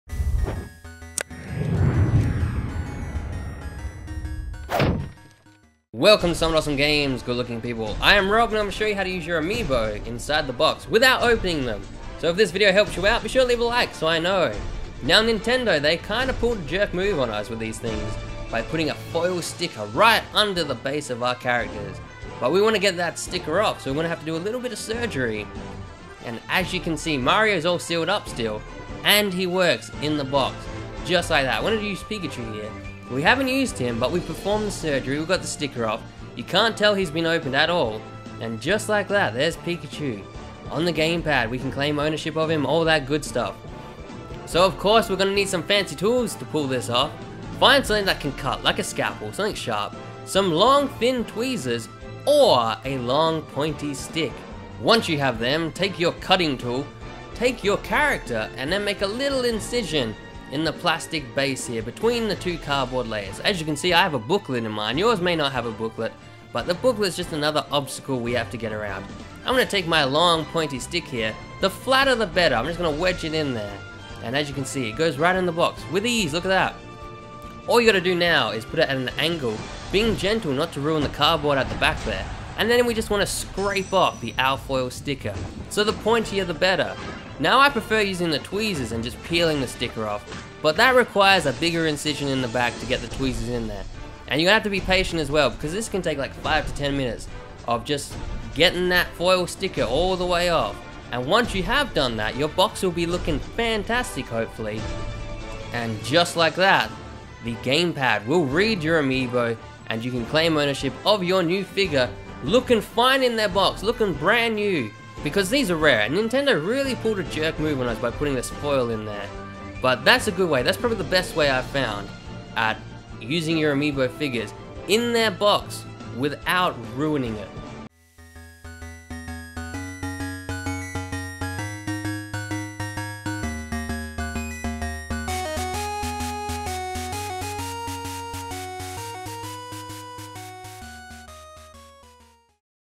How To Use Amiibos in the Box
Want to use your Amiibos in the box? Here is a good way to achieve that goal!

Not for 'Collectors' or fans of 'Mint Condition'. If that is you, good day sir.

Obviously these aren't for resale, that would be stupid, you would just leave them be and not use them. This is just for those who want to have them displayed looking fine in their awesome boxes and still use them.

Be sure to Comment, so we can have a chat! :D